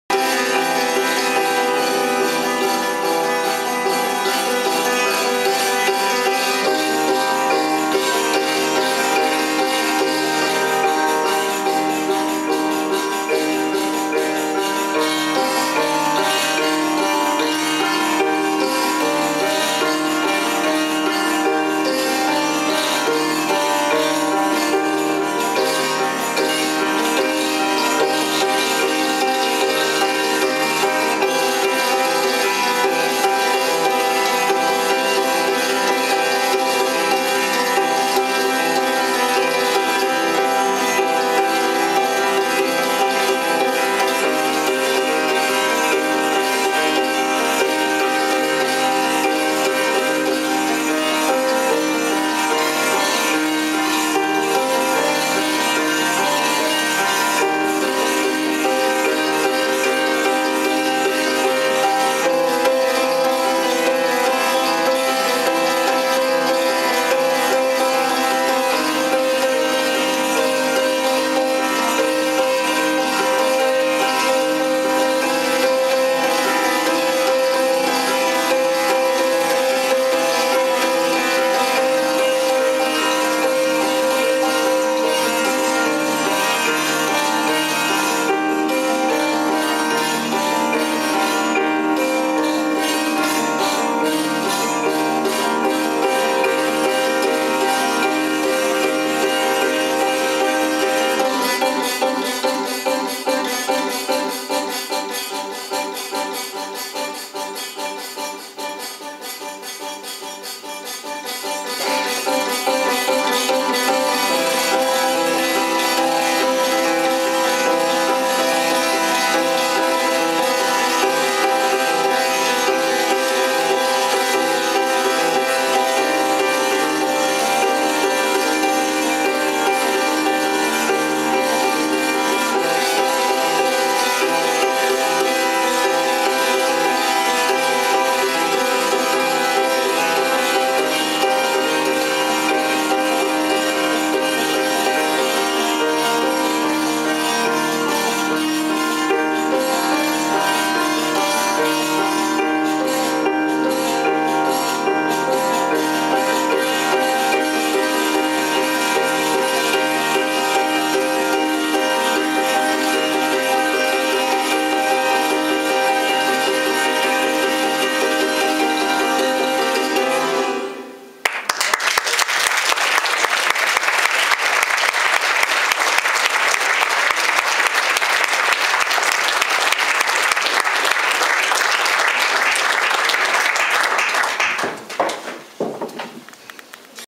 Bruno Sanfilippo Live in Bratislava Part 1/2
Piano prepared improvisation.
2 February 2013 · at Atelier babylon · Bratislava · Slovakia.